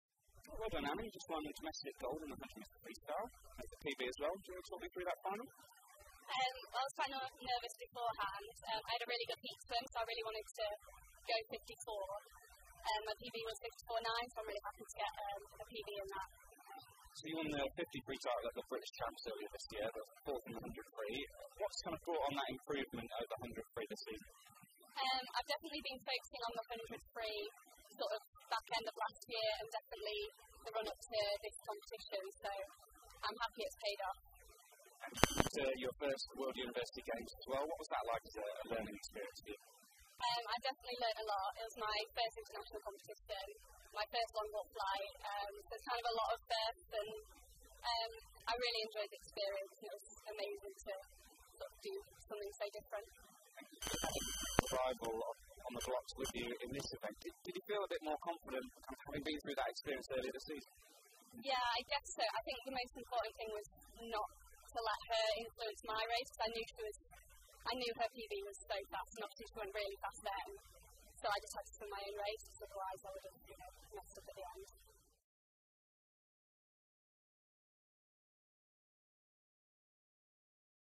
Anna Hopkin 100m Free Post Race Interview | Swim England National Winter Champs 2017
Anna Hopkin talks after winning the 100m Freestyle title at the Swim England National Winter Championships 2017 in Sheffield.

The Bath University swimmer set a PB 54.76 to take the touch on the penultimate night at Ponds Forge.